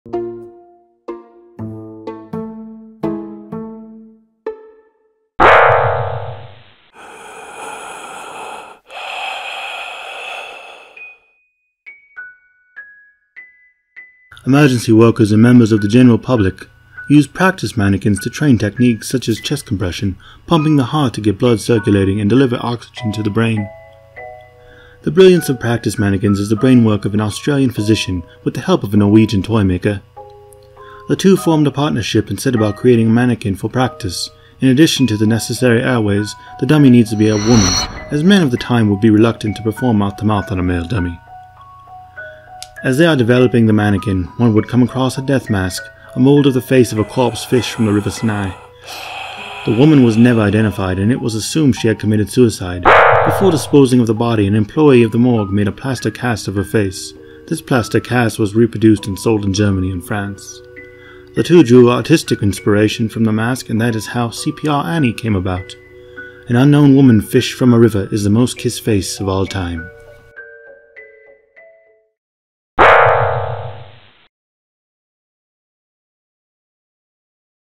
Cardiopulmonary Resuscitation Mannequin
Vintage child CPR mannequin and a brief history of the origin of Resusci Anne (CPR Annie).